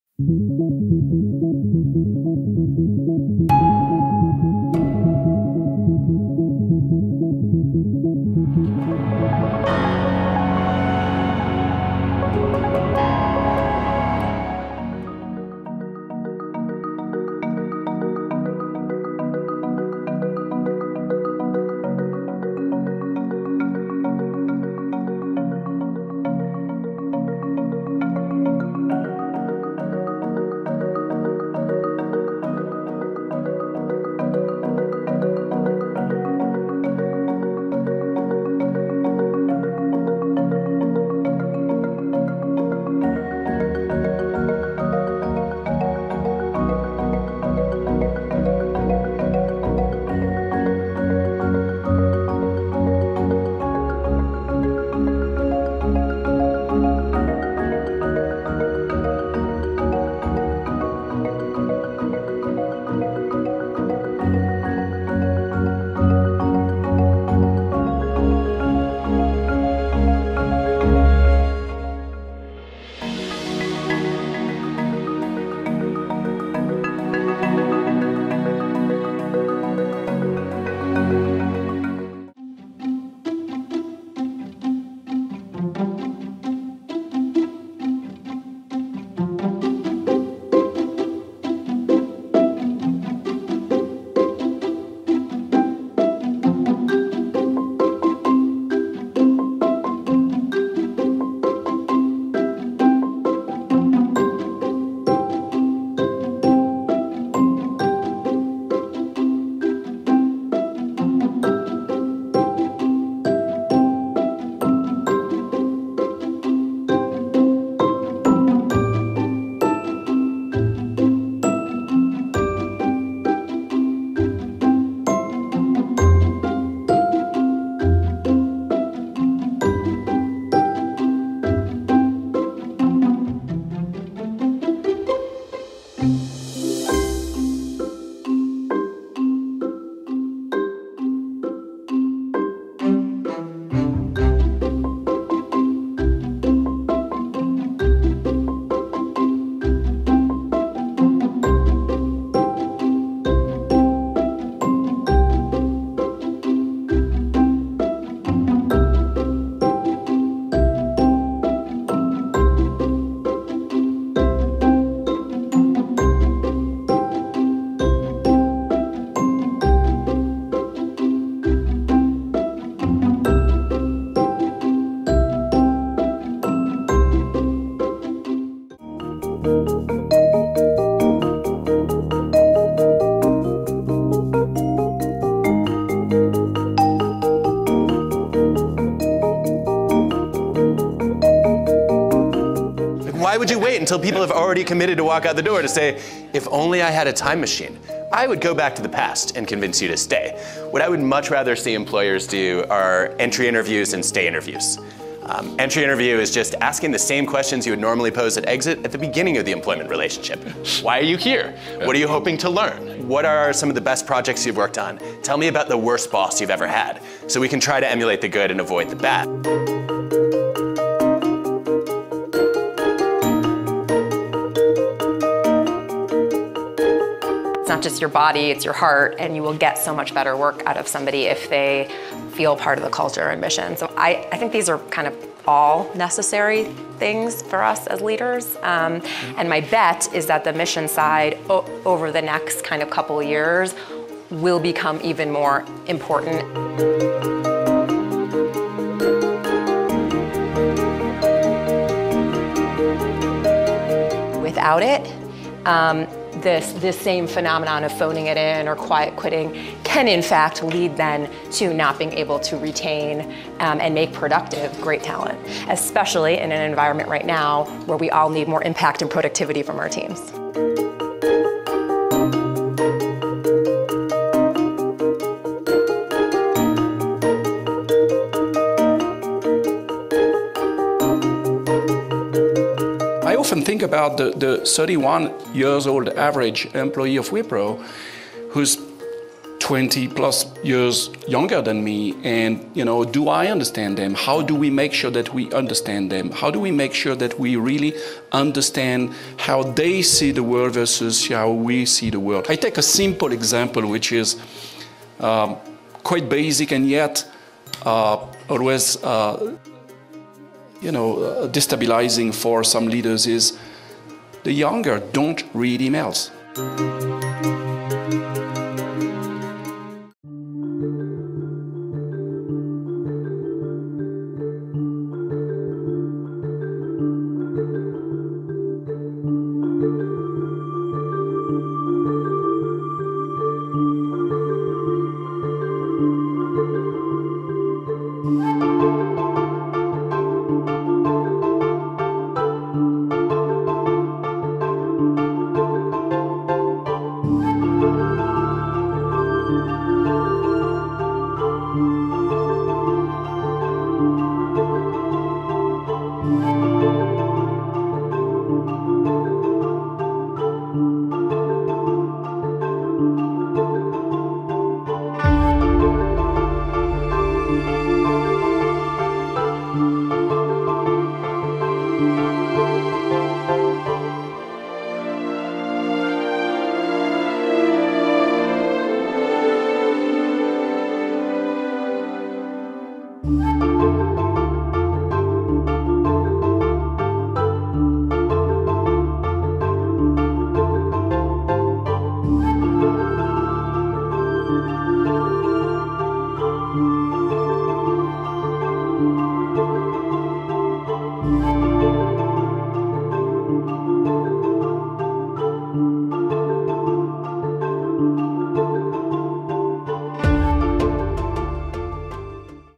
The AI robot changing farming & ChatGPT used for scientific research | WEF | Top Stories of the Week
This week's top stories of the week include:

0:15 This AI robot is revolutionizing farming - The robot can identify weeds and crops at rate of 20 plants a second. The robot then delivers either weedkiller or fertilizer directly to individual plants to an accuracy of within 1mm. It can treat up to 500,000 plants per hour. The robot was invented by US firm Verdant Robotics.

1:24 This company cancelled all internal meetings - In January, e-commerce platform Shopify cancelled all regular meetings with 3 people or more and imposed a 2-week window before staff could schedule anything new. It also permanently banned meetings on Wednesdays and limited large meetings to a strict window on Thursdays. More organizations are trying a ‘meeting reset’, from Dropbox to Asana and Zapier. But why - and could your organization try it too?

3:00 How to build a thriving workplace - Businesses often ask staff why they resign. So why not ask why they’re joining or staying?

"Why would you wait until people have already committed to walk out the door to say, ‘If only I had a time machine, I would go back to the past and convince you to stay’? What I would much rather see employers do are entry interviews and stay interviews. Entry interview is just asking the same questions you would normally pose at exit at the beginning of the employment relationship: Why are you here? What are you hoping to learn? Right. What are some of the best projects you've worked on? Tell me about the worst boss you've ever had so we can try to emulate the good and avoid the bad."

5:22 ChatGPT is already being used for scientific research - ChatGPT launched to the public in November 2022. It gives in-depth, natural language answers to prompts, based on what it has learned from a huge database of books and articles. People have been using ChatGPT to perform an impressive range of tasks, from writing university-level essays to debugging code and filling in job applications. It has written fake scientific reports that have fooled scientists and has even been used to write a children’s book.